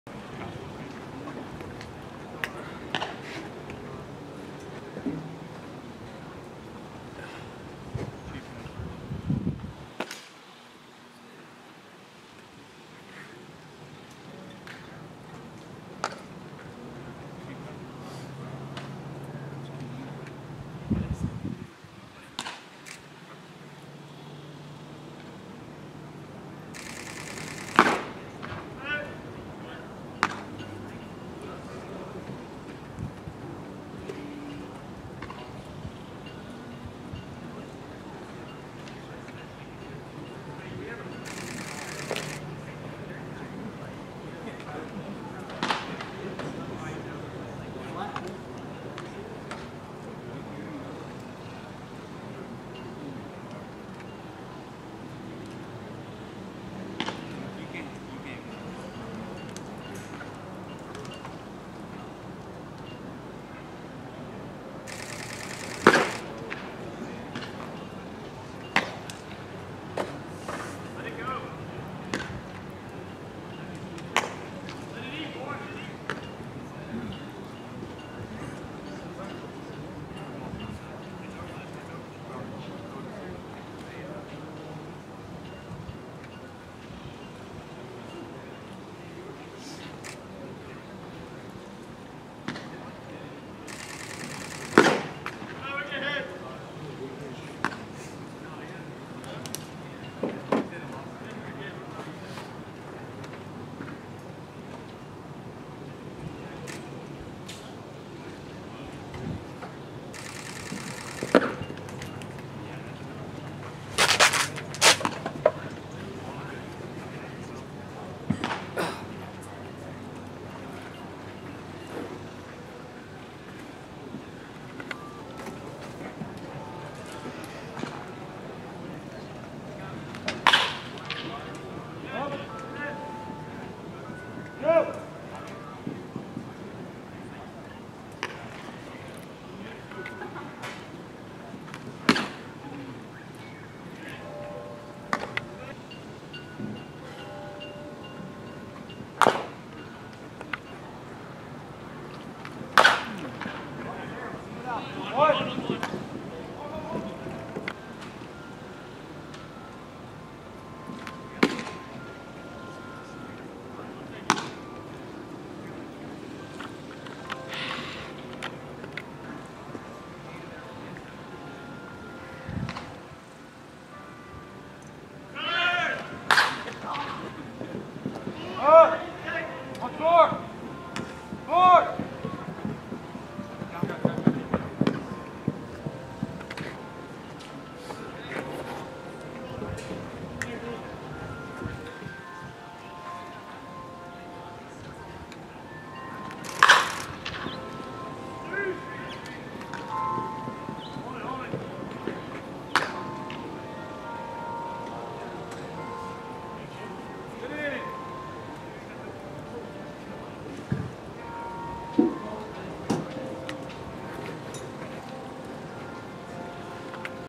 RHP Andrew Woods OUACHITA CHRISTIAN High School Class of 2021
Alabama Baseball Commit, RHP Andrew Woods OUACHITA CHRISTIAN High School Class of 2021 during the ACGames2020 at Dallas Baptist University.